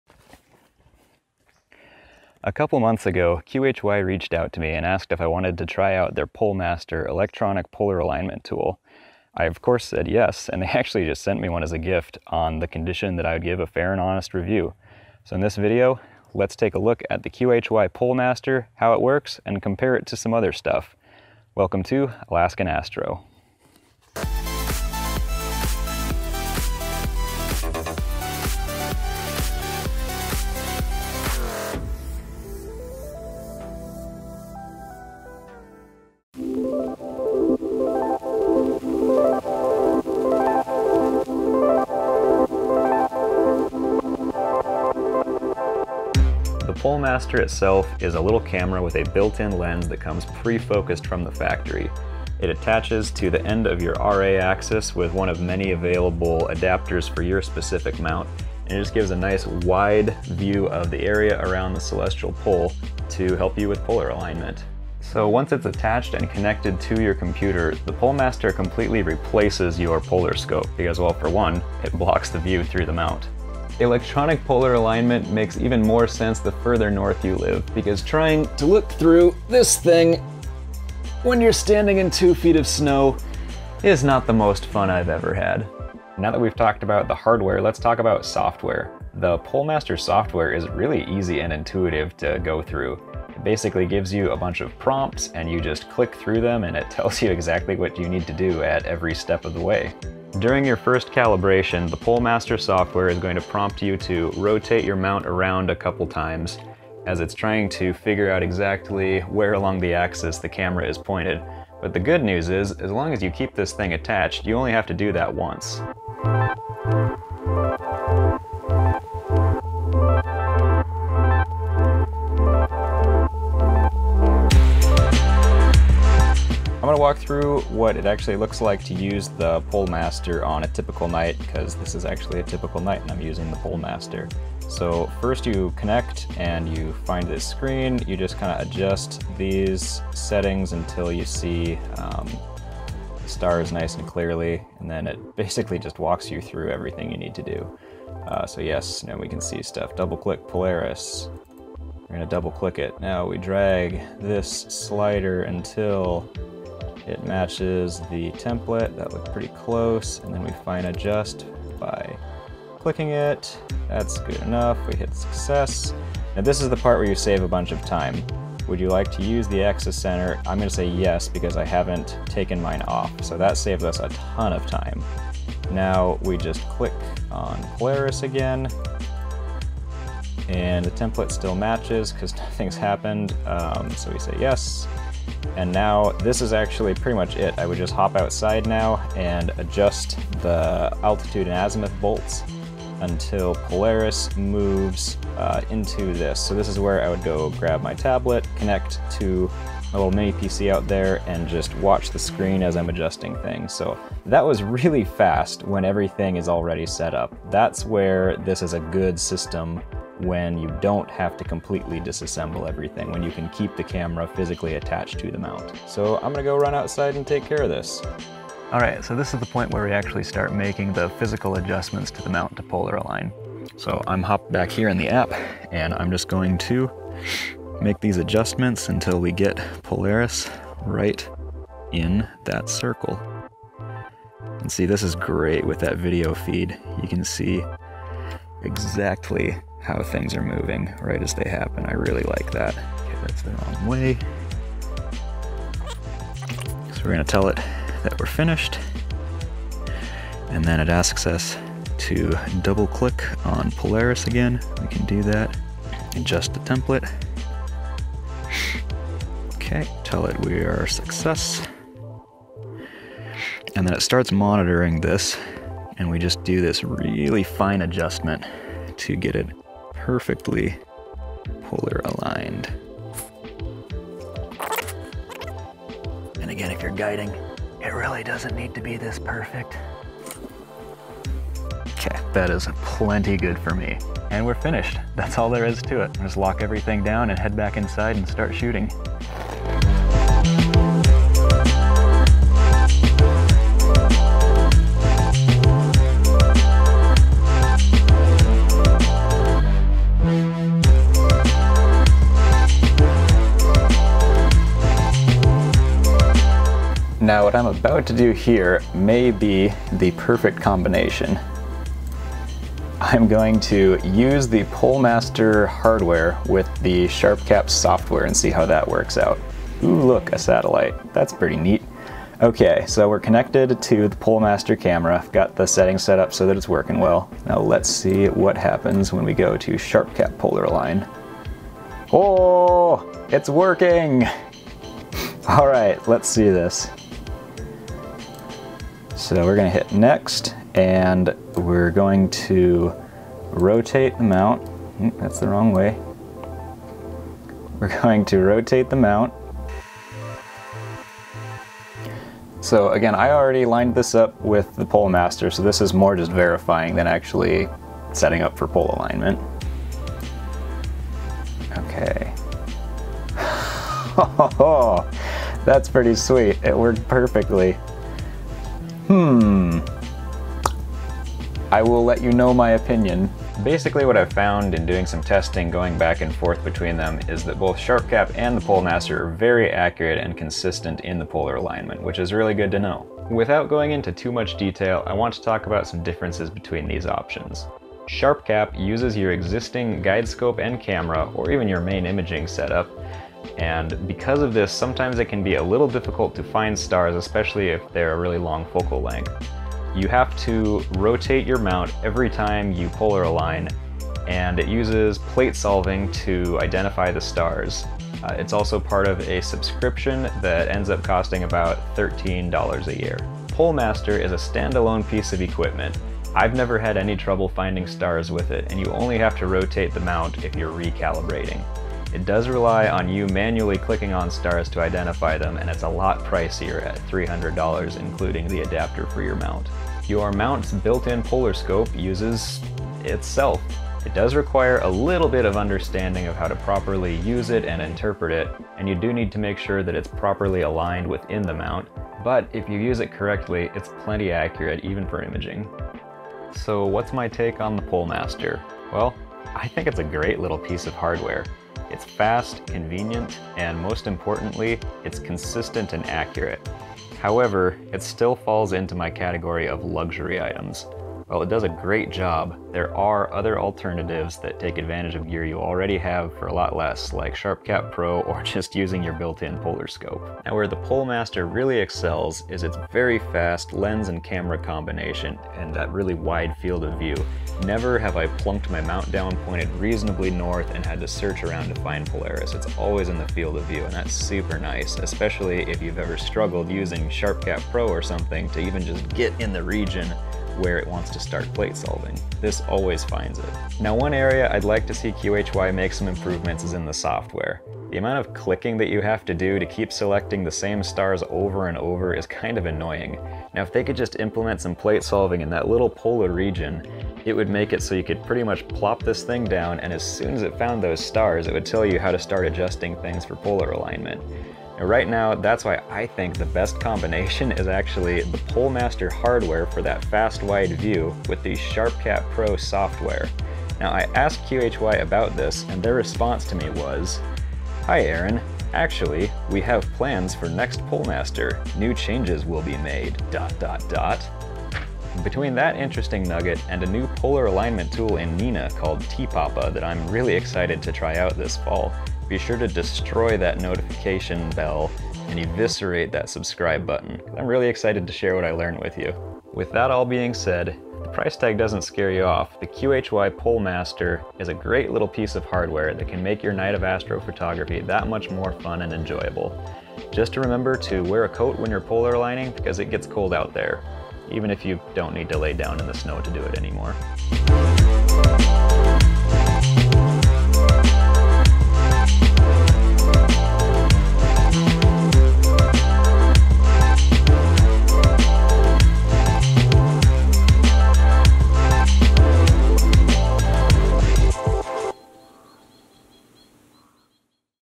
QHY PoleMaster Review and Comparison
QHY’s PoleMaster is a neat little piece of astrophotography equipment that makes polar aligning your equatorial mount quick, easy, and accurate. In this video we take a look at the hardware of the PoleMaster, how it pairs with the software to polar align your mount, and how it compares to using SharpCap Pro and a manual polar scope.

Timeline:
00:00
00:36 - Equipment
1:26 - Software
2:06 - Use in the Wild
6:13 - Comparison with SCP
7:25 - Conclusion

Equipment featured in this video:

QHY PoleMaster with EQ6 adapter
Orion Atlas EQ-G with Rowan belt mod and BoDai-Global Rail Kit
ASI1600MM-Pro (it’s old video)
ES HRCC
Optolong LRGBSHO Filters

Find full resolution images of M94 and Abell 21 on the AK Astro Flickr

Thank you to QHY for sending me this PoleMaster to use!